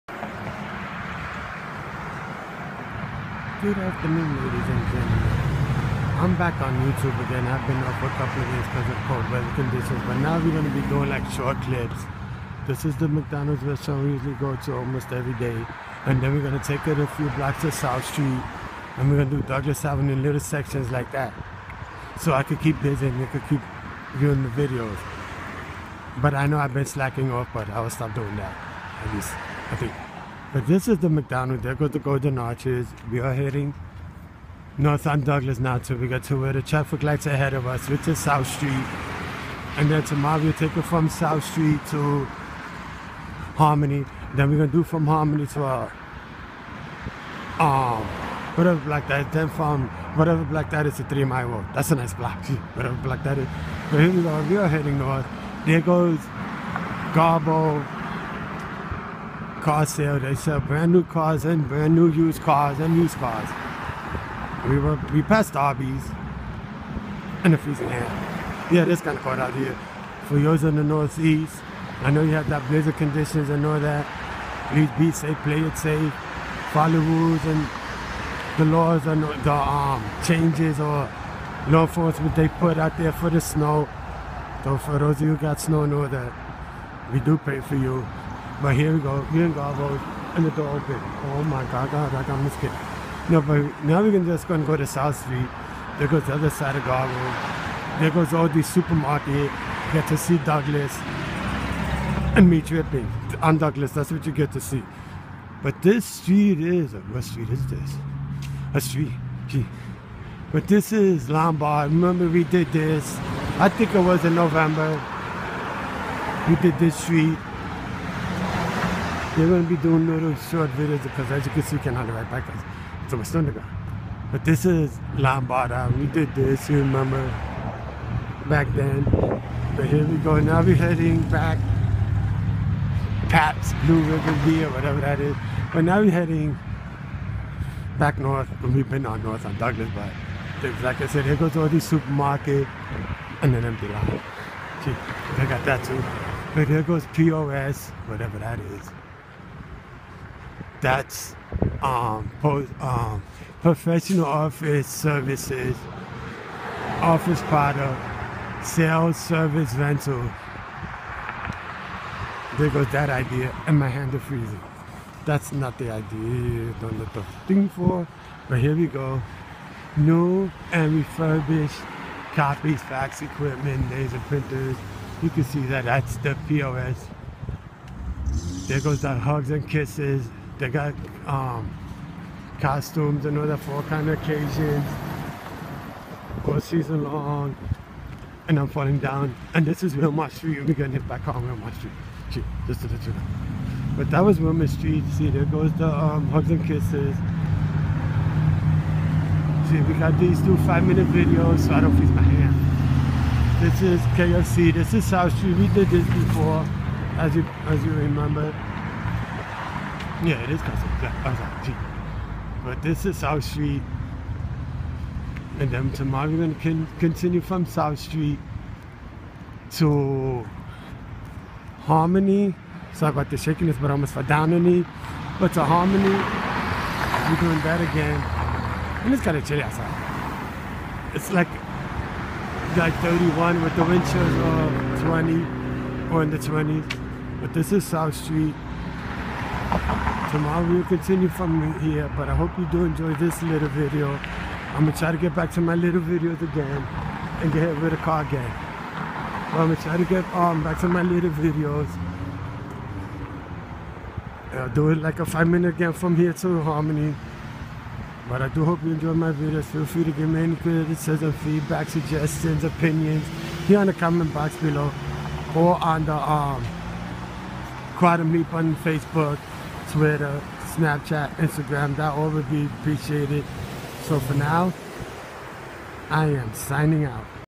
Douglas Ave. Part1
A walk on a section of Douglas Avenue from Wilmor Street to South Street.